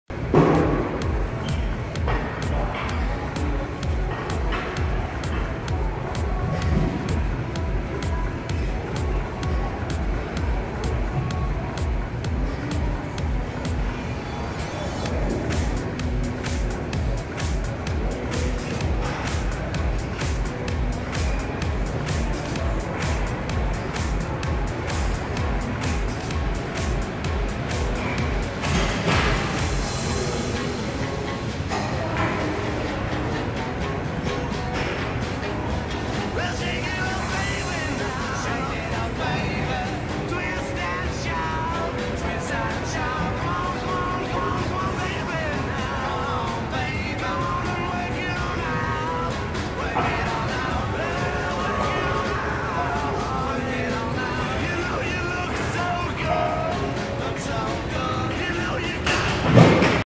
TRX Sidewinders (Spiral Line Mobilization)
Mobilize your spiral lines, spine, and pretty much everything in between.